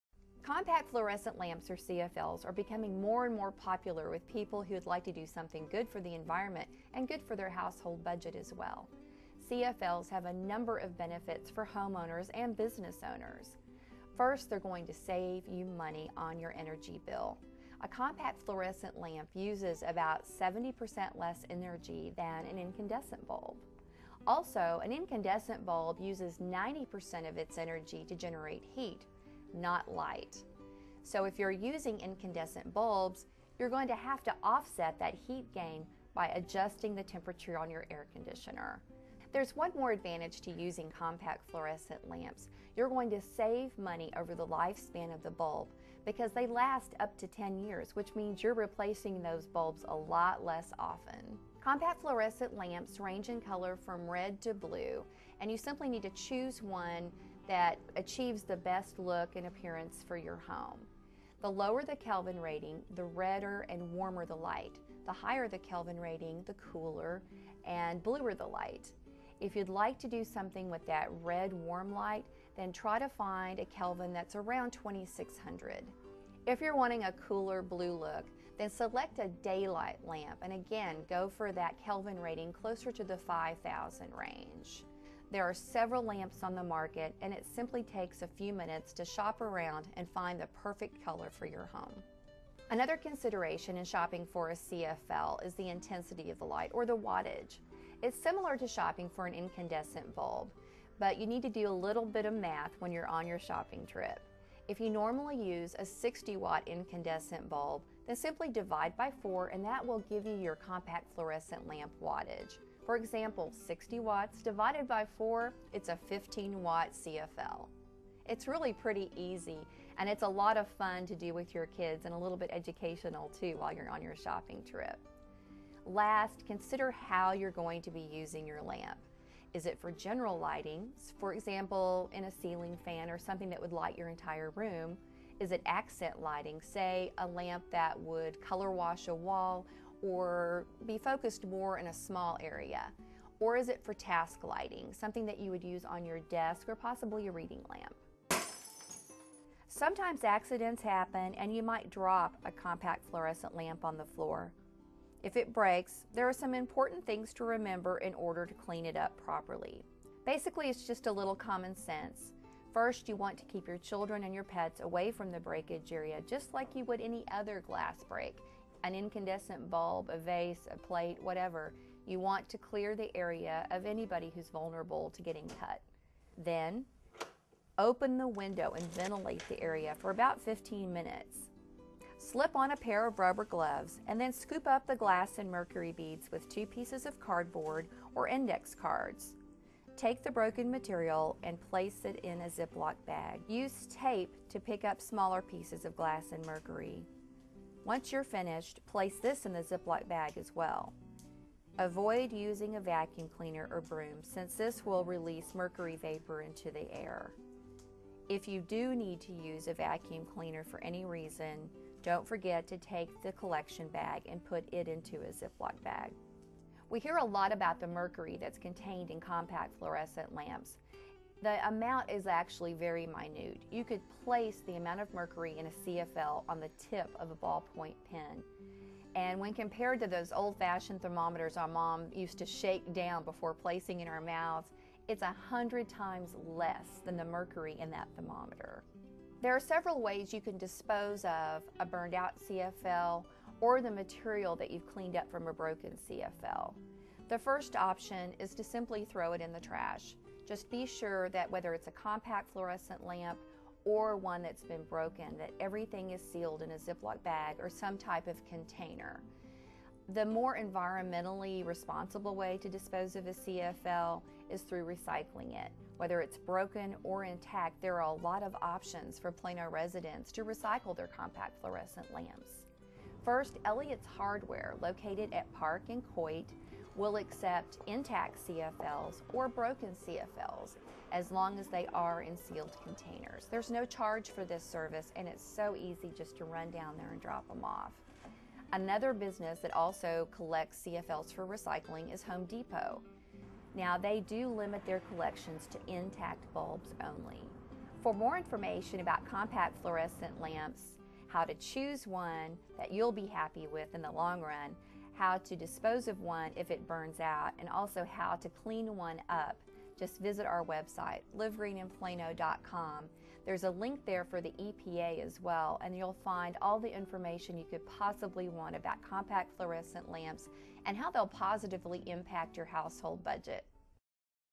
The Enlightening Basics Of CFLs
Produced by Plano Television Network wtih Melinda Haggerty. Learn how to choose the right CFL for the right use and clean up broken CFLs the safe, easy way.